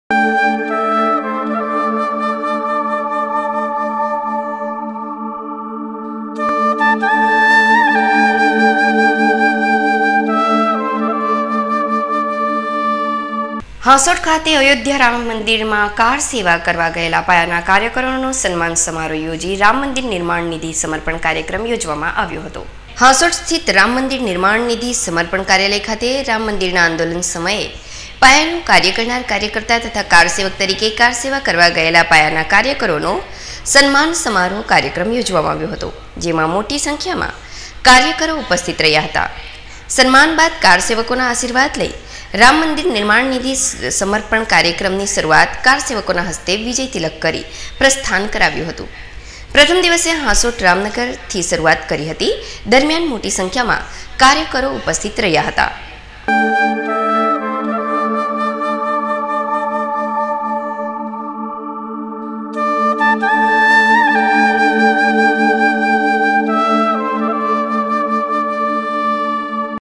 માઁ ટી.વી.25-01-2021
.હાંસોટમાં કારસેવા ગયેલ કાર્યકરોના સન્માન સાથે નિર્માણાધીન મંદિર માટે સમર્પણ કાર્યક્રમ યોજાયો
રામ મંદિર નિર્માણનિધિ  સમર્પણ કાર્યક્રમ અંતર્ગત કારો સેવકો હસ્તે વિજય તિલક કરી કાર્યકરો ફંડ  એકત્ર કરવામાં પ્રસ્થાન કરાવાયું